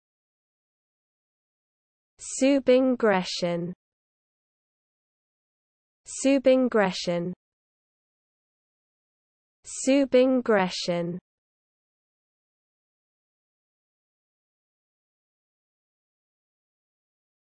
How to pronounce Subingression (CORRECTLY)
Learn how to correctly pronounce the word Subingression  in the US English accent.